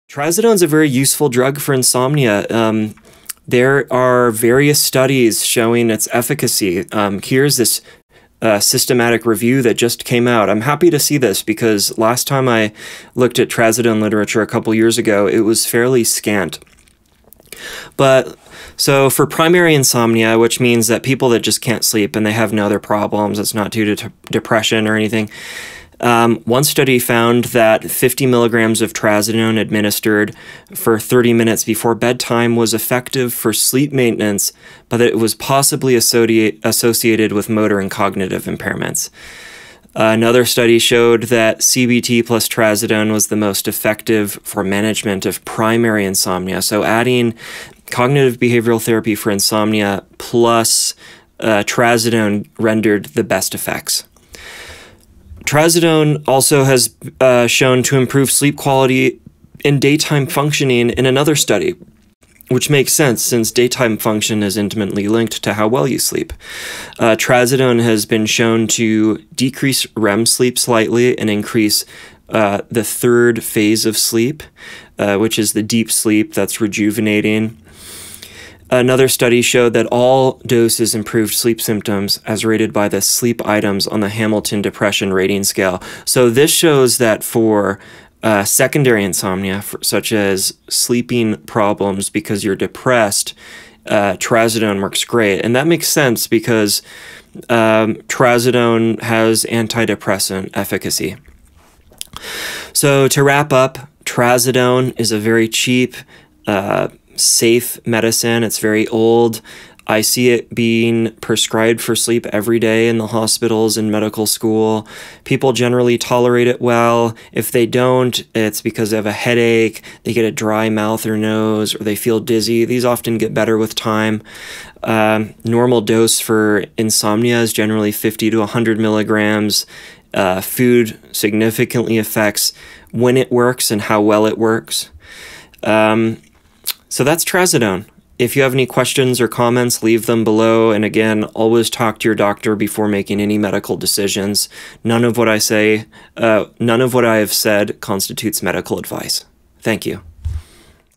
Trazodone for Insomnia Pt 3: The Evidence that it Works
In this video we go over the newest literature showing that Trazodone is effective for treating insomnia.